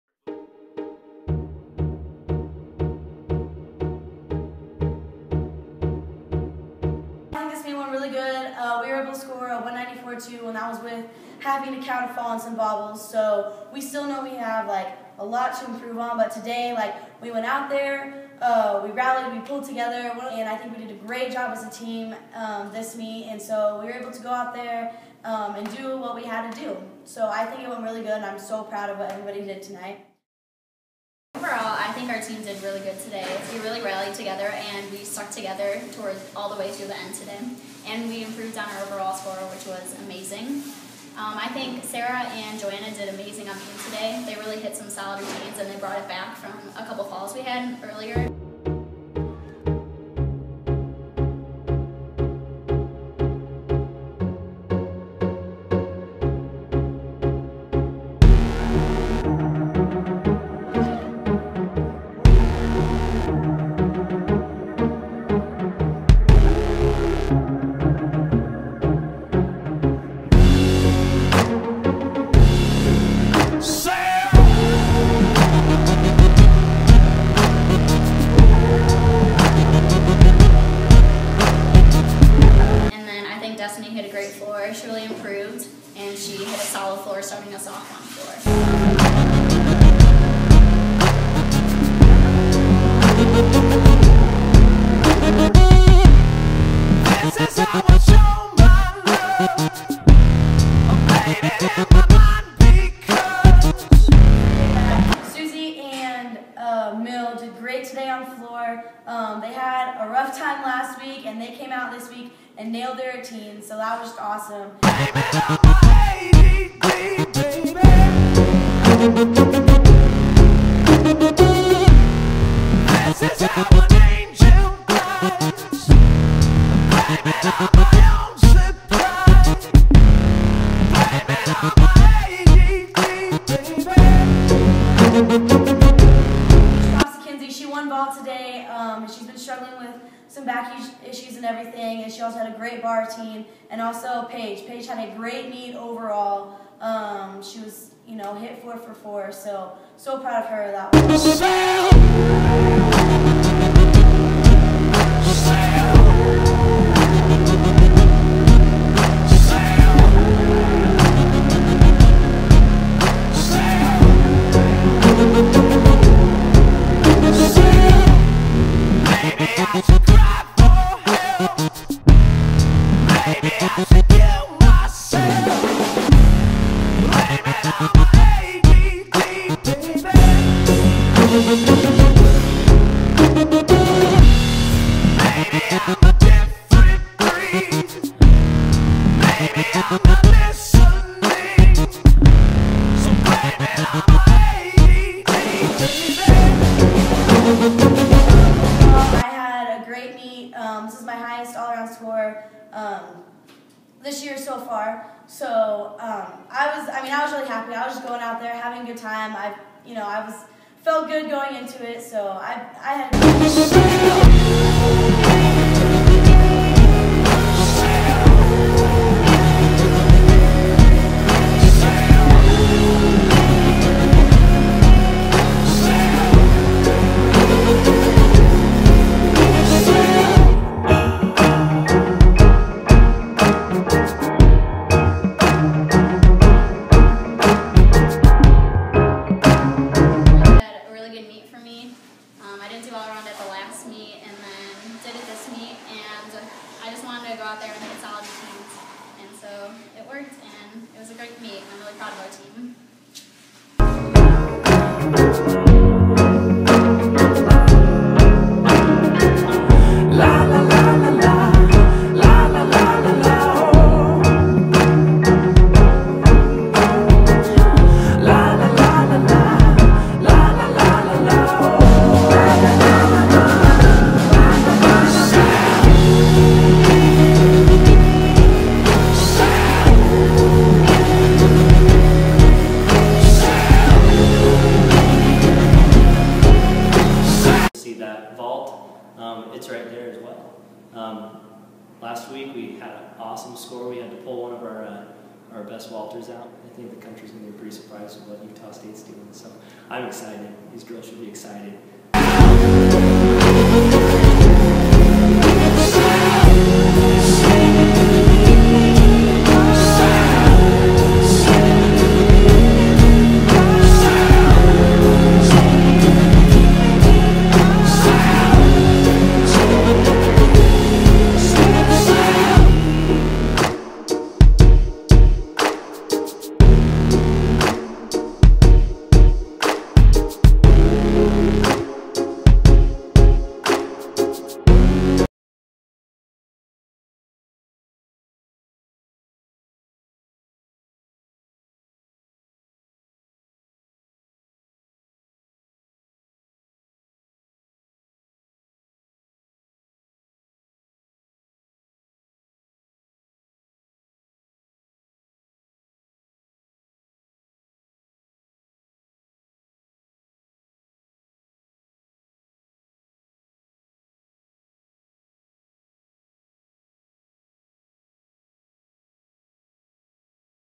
USU vs SUU '13
Featuring interviews from freshmen Hayley Sanzottie, sophmore Sarah Landes, and assistant coach David Tighe.